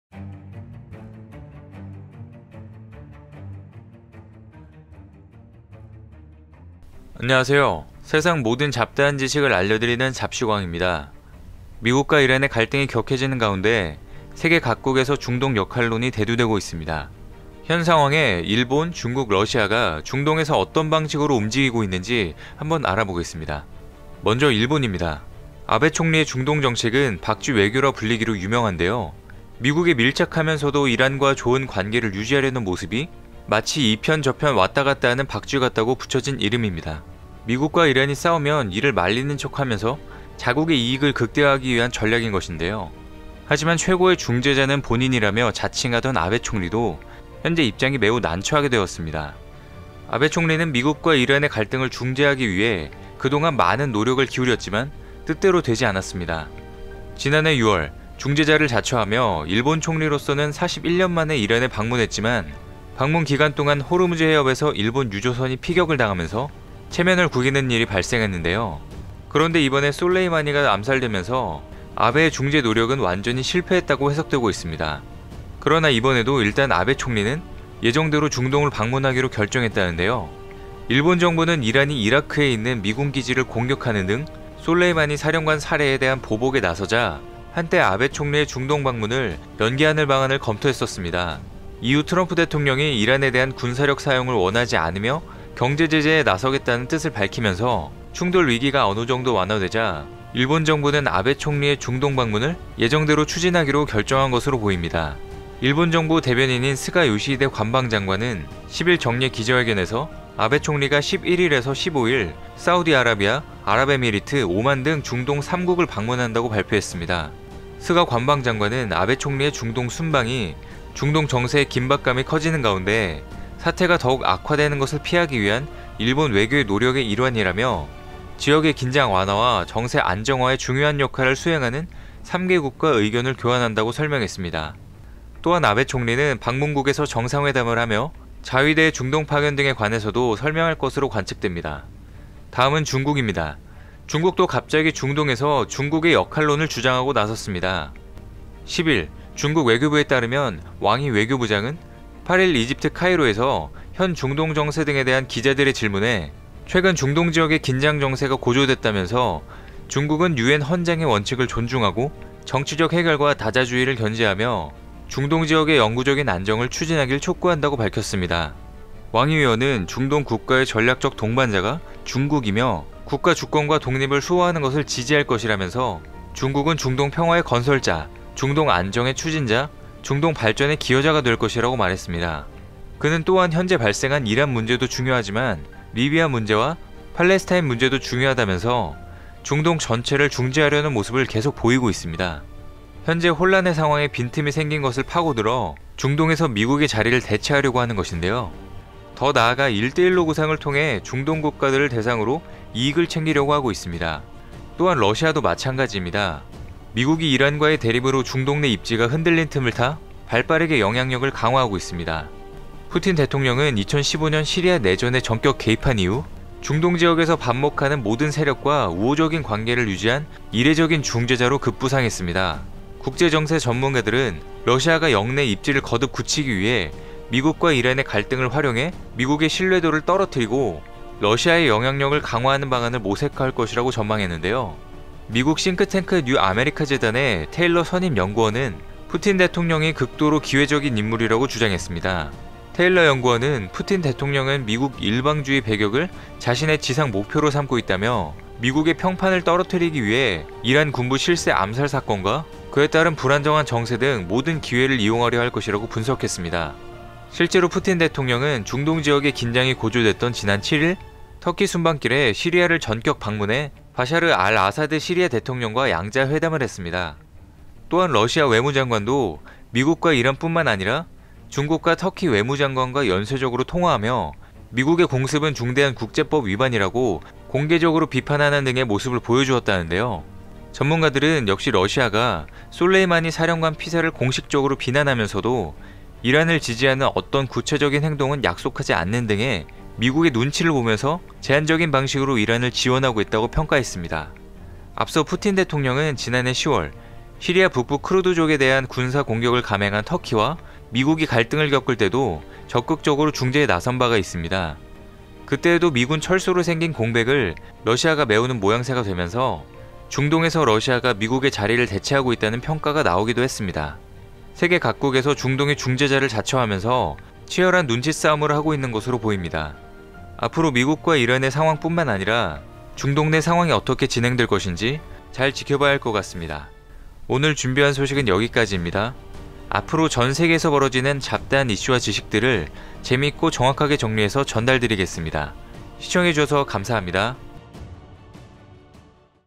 일본, 중국, 러시아가 다급히 중동으로 뛰어가는 이유 / 미국 눈치보며 각국이 치열하게 경쟁하는 상황 [잡식왕]
미국과 이란 갈등이 격해지는 가운데 세계 각국에서 중동 역할론이 대두되고 있습니다.
현상황에 일본, 중국, 러시아가 중동에서 어떤 방식으로 움직이고 있는지 한번 알아보겠습니다.

먼저 일본입니다.
아베 총리의 중동 정책은 '박쥐 외교'라 불리기로 유명한데요.

미국에 밀착하면서도 이란과도 좋은 관계를 유지하려는 모습이
마치 이편저편 왔다갔다하는 박쥐 같다고 붙여진 이름입니다.

미국과 이란이 싸우면 이를 말리는 척하면서
자국 이익을 극대화하기 위한 전략인것인데요.

하지만 최고의 중재자는 본인이라며 자칭하던
아베 총리도 현재 입장이 매우 난처하게 되었습니다.

아베 총리는 미국과 이란 갈등을 중재하기 위해
그동안 많은 노력을 기울였지만 뜻대로 되지 않았습니다.

지난해 6월 중재자를 자처하며
일본 총리로 41년만에 이란에 방문했지만,

방문 기간 동안 호르무즈 해협에서 일본 유조선이 피격을 당하면서
체면을 구기는일이 발생했는데요.

그런데 이번에 솔레이마니가 암살되면서 아베의
중재 노력은 완전히 실패했다고 해석되고 있습니다.

그러나 이번에도 일단 아베 총리는 예정대로
중동을 방문하기로 결정했다는데요.

일본 정부는 이란이 이라크에 있는 미군 기지를 공격하는 등
솔레이마니 사령관 살해에 대한 보복에 나서자

한때 아베 총리의 중동 방문을
연기하는 방안을 검토했었습니다.

이후 트럼프 대통령이 이란에 대한 군사력 사용을
원하지 않으며 경제 제재에 나서겠다는 뜻을 밝히면서

충돌 위기가 어느정도 완화되자 일본 정부는 아베 총리의 중동 방문을
예정대로 추진하기로 결정한 것으로 보입니다.

일본 정부 대변인인 스가 요시히데 관방장관은
10일 정례기자회견에서 아베 총리가 11∼15일

사우디아라비아, 아랍에미리트(UAE), 오만 등
중동 3국을 방문한다고 발표했습니다.

스가 관방장관은 아베 총리의 중동 순방이
"중동 정세의 긴박감이 커지는 가운데

사태가 더욱 악화하는 것을 피하기 위한
일본 외교 노력의 일환"이라며

"지역의 긴장 완화와 정세 안정화에 중요한 역할을
수행하는 3개국과 의견 교환을 한다"고 설명했습니다.

또한 아베 총리는 방문국에서 정상회담을 하며
자위대의 중동 파견 등에 관해서도 설명할 것으로 관측됩니다.

다음은 중국입니다.
중국도 갑자기 중동에서 중국의 역할론을 주장하고 나섰습니다.

10일 중국 외교부에 따르면 왕이 외교부장은
8일 이집트 카이로에서 현 중동 정세 등에 대한 기자들의 질문에

"최근 중동 지역의 긴장 정세가 고조됐다"면서
"중국은 유엔 헌장의 원칙을 존중하고

정치적 해결과 다자주의를 견지하면서
중동 지역의 영구적인 안정을 추진하길 촉구한다"고 밝혔습니다.

왕이 위원은 중동 국가의 전략적 동반자가 중국이며
"국가 주권과 독립을 수호하는 것을 지지할 것"이라면서

"중국은 중동 평화의 건설자, 중동 안정의 추진자,
중동 발전의 기여자가 될 것"이라고 말했습니다.

그는 또한 현재 발생한 이란 문제도 중요하지만
리비아 문제와 팔레스타인 문제도 중요하다면서
중동 전체를 중재를 하려는 모습을 계속 보이고 있습니다.

현재 혼란의 상황에 빈틈이 생긴것을 파고들어
중동에서 미국의 자리를 대체하려고 하는것인데요.

더 나아가 일대일로 구상을 통해
중동 국가들을 대상으로 이익을 챙기려고 하고 있습니다.

또한 러시아도 마찬가지 입니다.
미국이 이란과의 대립으로 중동 내 입지가
흔들린 틈을 타 발 빠르게 영향력을 강화하고 있습니다.

푸틴 대통령은 2015년 시리아 내전에 전격 개입한 이후,

중동 지역에서 반목하는 모든 세력과 우호적인
관계를 유지한 '이례적인 중재자'로 급부상했습니다.

국제 정세 전문가들은 러시아가 역내 입지를 거듭 굳히기 위해
미국과 이란의 갈등을 활용해 미국의 신뢰도를 떨어뜨리고,

러시아의 영향력을 강화하는 방안을 모색할 것이라고 전망했는데요.

미국 싱크탱크 뉴아메리카재단의 테일러 선임 연구원은
푸틴 대통령이 극도로 기회주의적인 인물이라고 주장했습니다.

테일러 연구원은 "푸틴 대통령은 미국의 일방주의
배격을 자신의 지상 목표로 삼고 있다"며

"미국의 평판을 떨어뜨리기 위해 이란 군부실세 암살 사건과
그에 따른 불안정한 정세 등 모든 기회를
이용하려 할 것"이라고 분석했습니다.

실제로 푸틴 대통령은 중동 지역의 긴장이 고조됐던 지난 7일
터키 순방길에 시리아를 전격 방문해
바샤르 알아사드 시리아 대통령과 양자 회담을 했습니다.

또한 러시아 외무장관도 미국과 이란뿐만 아니라,
중국과 터키 외무장관과 연쇄적으로 통화하며

"미국의 공습은 중대한 국제법 위반"이라고
공개적으로 비판하는 등의 모습을 보여주었다는데요.

전문가들 역시 러시아가 솔레이마니 사령관 피살을
공식적으로 비난하면서도 이란을 지지하는 어떤 구체적인 행동은

약속하지 않는 등 미국의 눈치를 보며
제한적인 방식으로 이란을 지원하고 있다고 평가했습니다.

앞서 푸틴 대통령은 지난해 10월 시리아 북부 쿠르드족에 대한
군사 공격을 감행한 터키와 미국이 갈등을 겪을 때도
적극적으로 중재에 나선 바가 있습니다.

그때에도 미군 철수로 생긴 공백을 러시아가 메우는 모양새가 되면서
중동에서 러시아가 미국의 자리를 대체하고 있다는 평가가 나오기도 했습니다.

세계 각국에서 중동의 중재자를 자처하면서
치열한 눈치 싸움을 하고 있는 것으로 보입니다.

앞으로 미국과 이란의 상황 뿐만아니라
중동내 상황이 어떻게 진행될것인지 잘 지켜봐야할 것 같습니다.

오늘 준비한 소식은 여기까지입니다.
앞으로도 전 세계에서 벌어지는 잡다한 이슈와 지식들을
재미있고 정확하게 정리해서 전달드리겠습니다.

연합뉴스 컨텐츠 관련 부분인용 및 전체인용에 대한 허가를 받았습니다.